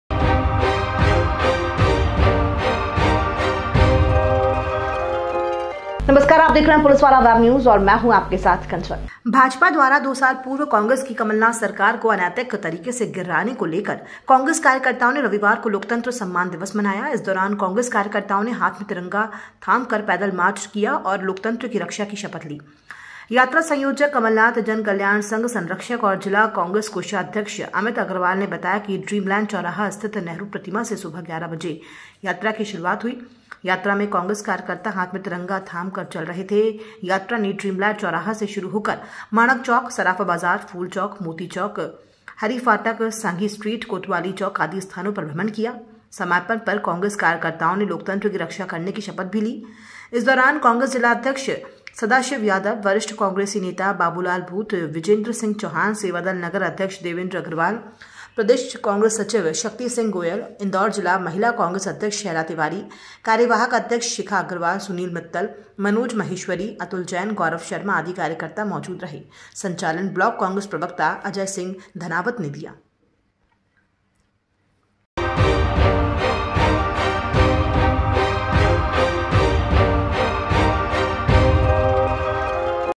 कमलनाथ सरकार काे अनैतिक तरीके से गिराने काे लेकर कांग्रेस कार्यकर्ताओ ने लाेकतंत्र सम्मान दिवस मनाया
कमलनाथ सरकार काे अनैतिक तरीके से गिराने काे लेकर कांग्रेस कार्यकर्ताओ ने रविवार काे लाेकतंत्र सम्मान दिवस मनाया- 21 मार्च